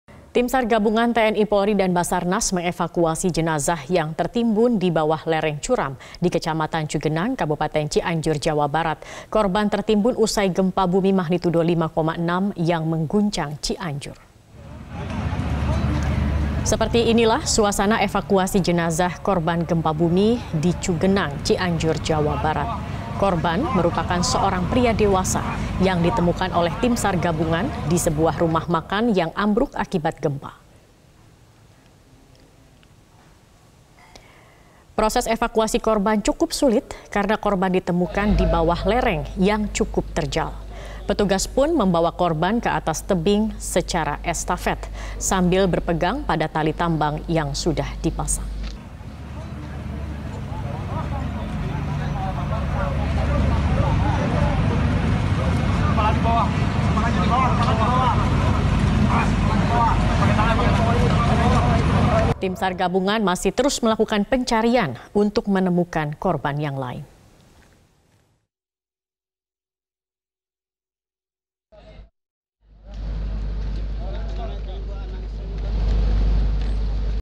Tim SAR Gabungan Evakuasi Satu Korban Gempa Cianjur di Lereng Curam
MetroTV, Tim SAR gabungan TNI-Polri dan Basarnas mengevakuasi satu jenazah korban gempa Cianjur, Jawa Barat, yang tertimbun di bawah lereng curam di Kecamatan Cugenang.

Korban merupakan seorang pria dewasa yang ditemukan oleh tim SAR gabungan di sebuah rumah makan yang ambruk akibat gempa. Proses evakuasi korban cukup sulit karena korban ditemukan di bawah lereng yang cukup curam.

Petugas pun membawa korban ke atas tebing secara estafet sambil berpegang pada  tali tambang yang sudah dipasang.